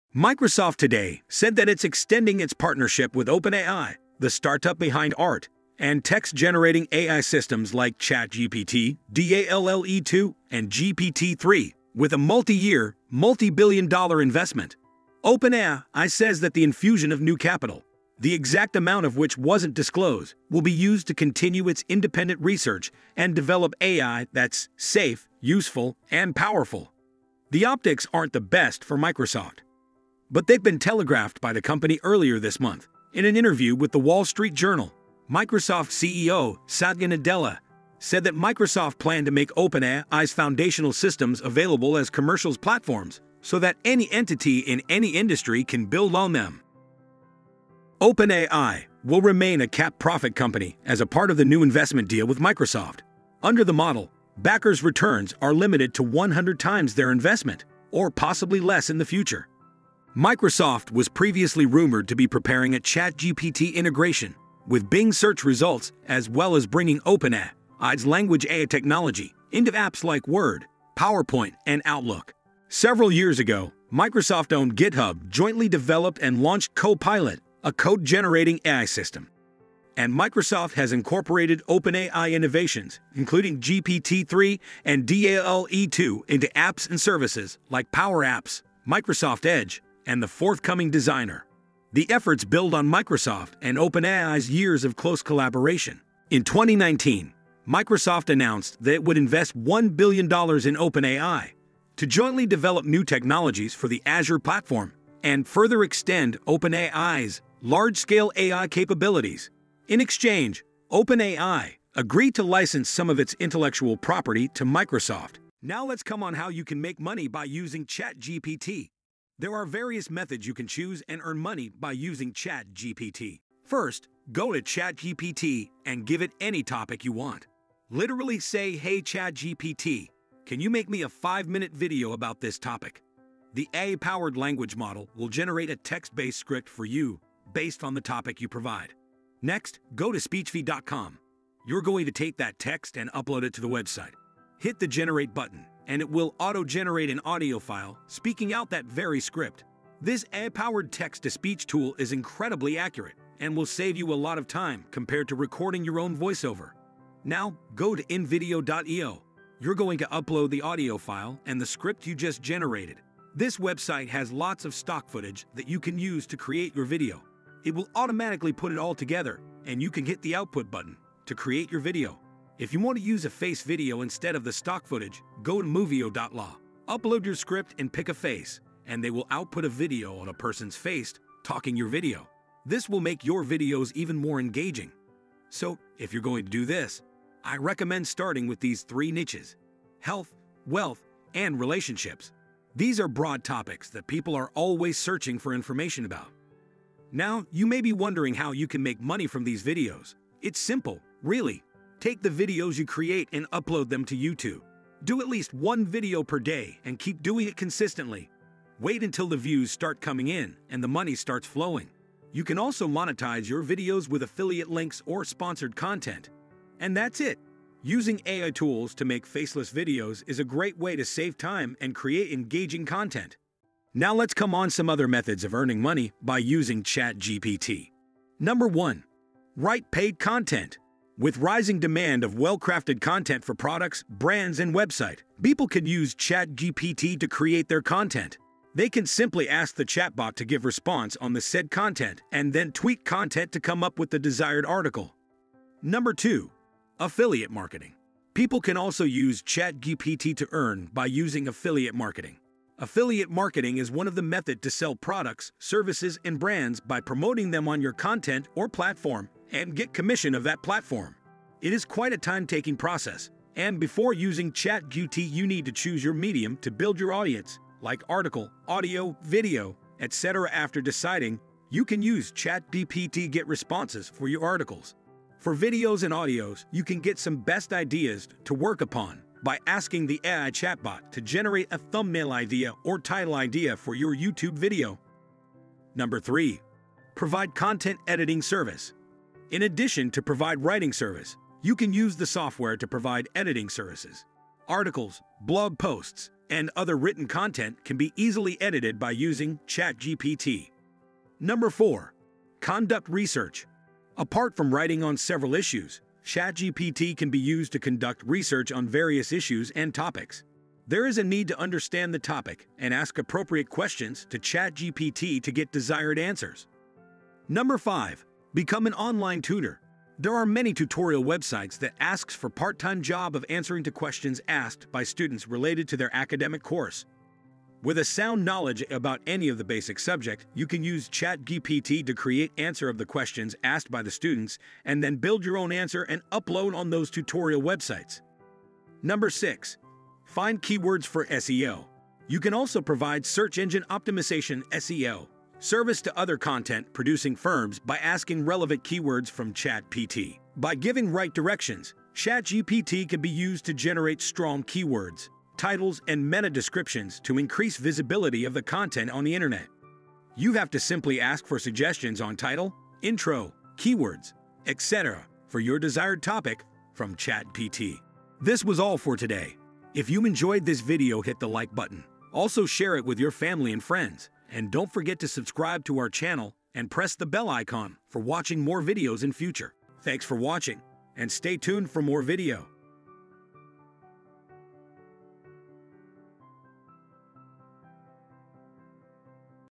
Microsoft and Chatgpt Investment Deal✨ Easy Ways to Make Money with Chatgpt
Hey Guys! Welcome! In today’s video, I’ll discuss about Microsoft and Chatgpt Investment deal, also I’ll show easy ways to make money with Chatgpt.

In January 2023, Microsoft announced a $10 billion investment in OpenAI, the company behind the artificial intelligence chatbot ChatGPT. The deal made Microsoft OpenAI's exclusive cloud service provider and gave the company access to OpenAI's cutting-edge AI research.

The investment is a major coup for Microsoft, looking to catch up to Google and Amazon in the race to develop artificial general intelligence (AGI). ChatGPT is one of the most advanced AGI systems in the world, and its capabilities will only improve with Microsoft's investment.

Microsoft plans to use ChatGPT to power various products and services, including the Bing search engine, the Office productivity suite, and the Azure cloud platform. The company also sees ChatGPT as a potential platform for new products and services, such as virtual assistants and chatbots.

The deal between Microsoft and OpenAI is a significant development in the AI landscape. It shows that Microsoft is serious about AI and willing to invest heavily in the technology. It also puts Microsoft in a solid position to compete with Google and Amazon to develop AGI.
Also, if you are looking for ways to make money using technology, ChatGPT is the right platform.

With AI technologies like ChatGPT, there are more ways than ever to make money from your skills and knowledge. From creating content to helping customers, the options are endless.

ChatGPT (Chat Generative Pre-trained Transformer) is a chatbot launched by OpenAI in November 2022. The tool lets you type natural-language prompts from a power player in artificial intelligence called OpenAI. ChatGPT offers conversational, if somewhat stilted, responses and answers your questions.

ChatGPT's primary use is in human-like conversational simulation, but it can do much more. You can take furniture ideas from it and apply them to your work.

This AI-based technology has gained popularity over time in just a few months, and there has been much discussion. Witnessing its success and popularity, many have stated that ChatGPT will end the future of Google.

This AI-powered chatbot not only gives accurate answers to the users, but you could also make money from it. We have put together 6 ways to earn money using ChatGPT in 2023.

▶️ Here are top 6 ways you can earn money using ChatGPT:
1. Write Paid Content
3. Provide Content Editing Service
4. Conduct Research
5. Become an online tutor
6. Find Keywords for SEO